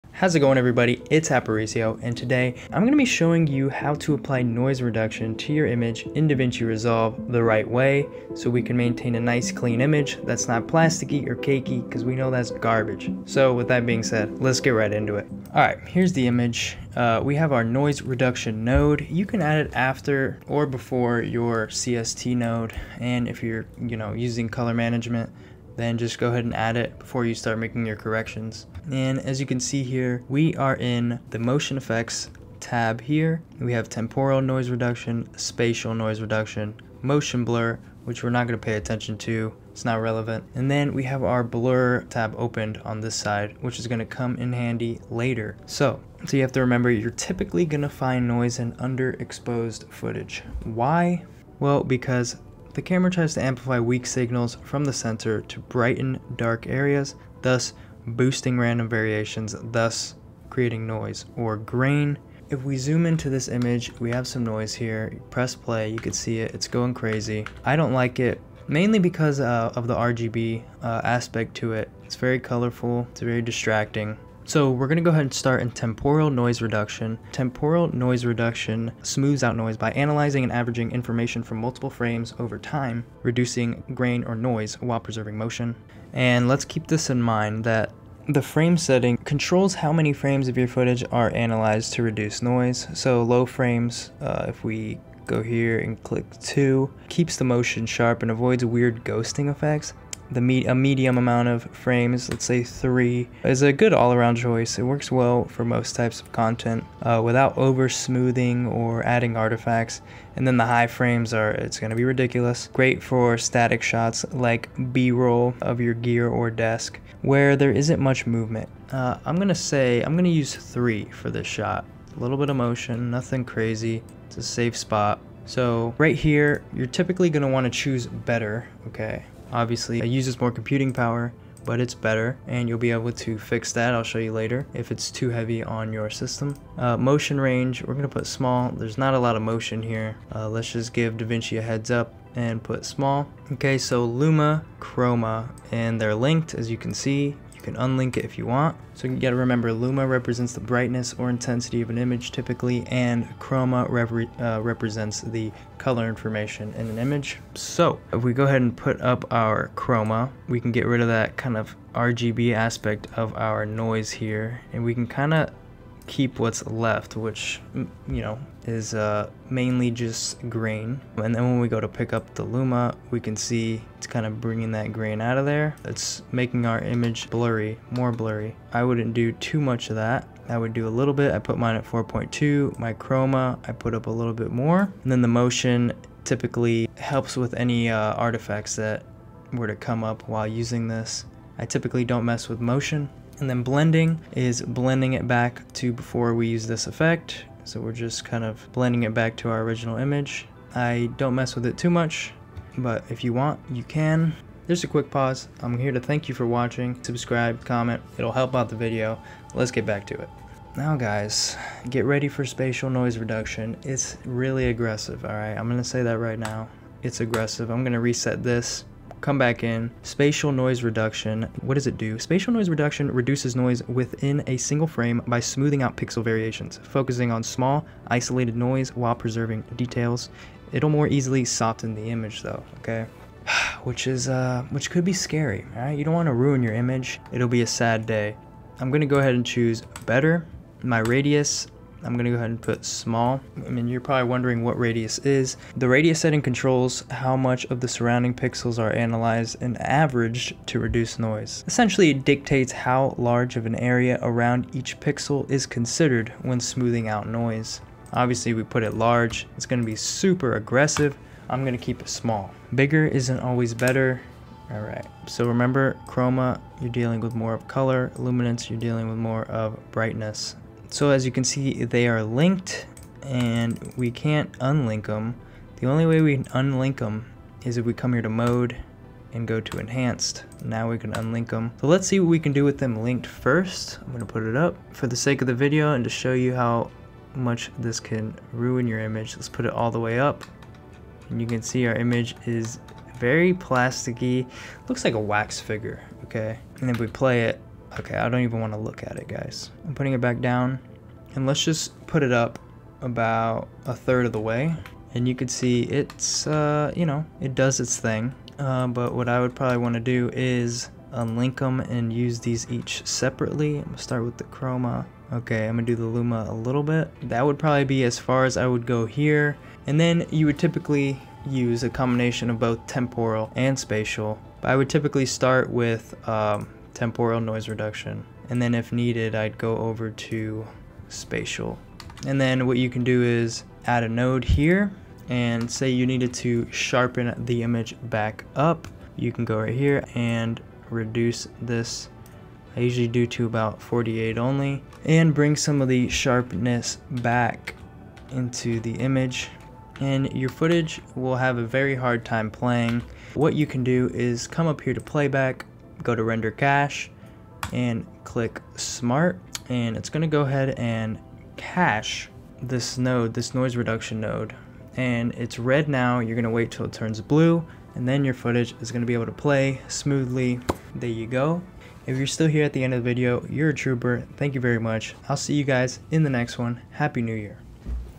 Reduce NOISE like a PRO in Davinci Resolve! | Simple Tutorial
In this video I go over how to reduce noise in davinci resolve without overdoing it, so you can maintain a nice clean image that does not come across as plastic like, or caky. This method can only be used in the studio version of davinci resolve. it is important not to overdo this technique so we can still have natural texture left in the image.

TIMESTAMPS:
0:00 Intro
0:15 Shot Overview
1:14 Temporal NR
3:30 Spatial NR
5:34 Sharpening
5:49 Smooth Playback
6:11 Outro